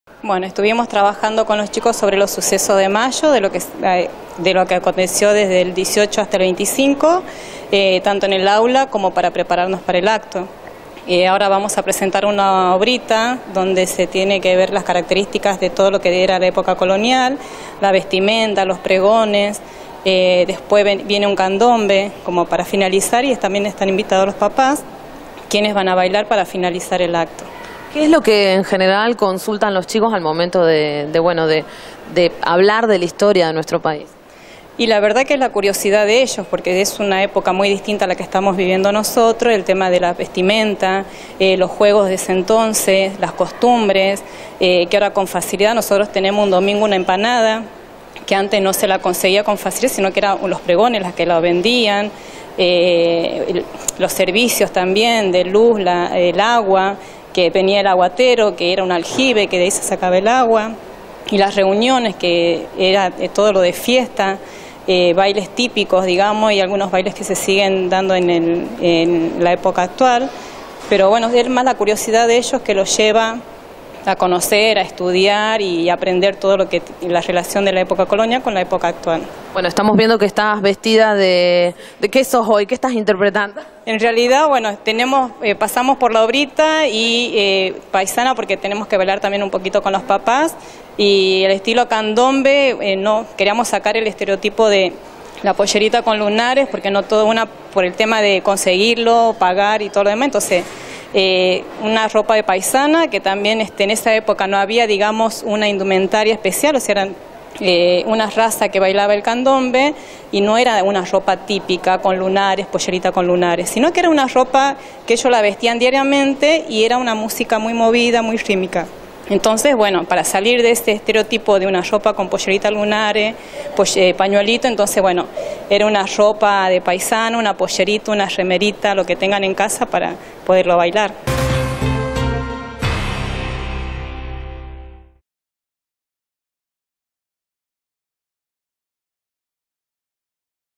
Festejos 25 de mayo | Actualidad | 24/05/2018 - TV2 Noticias
La comunidad educativa de la Escuela Nº1 realizó el acto para festejar la Revolución de Mayo